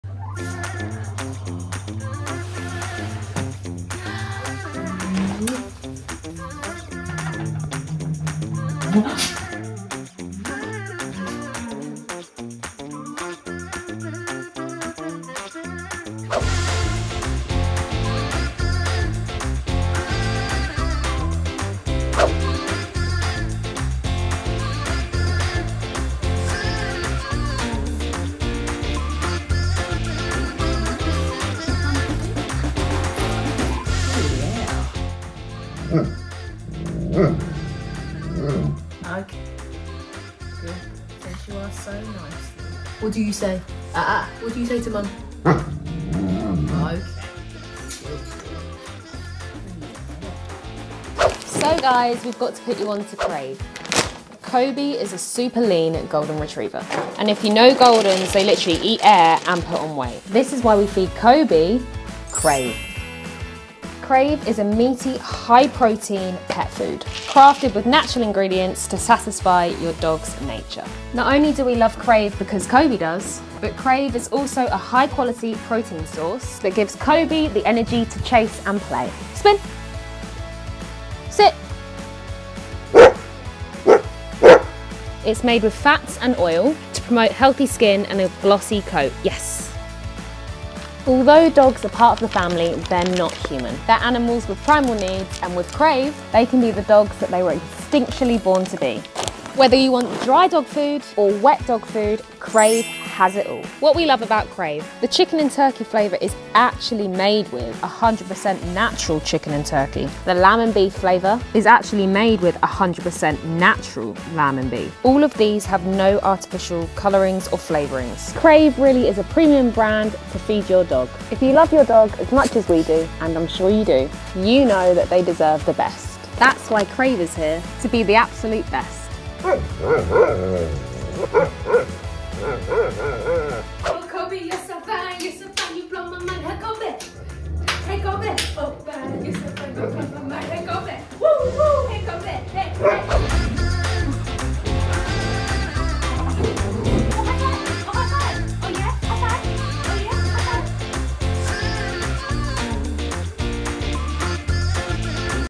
Why My Golden Retriever Loves My Girlfriend
Kobe has gotten on with Ella since the day they met, you can see why she has become Kobe’s best friend. Kobe has unconditional love for my girlfriend and it' amazing to see.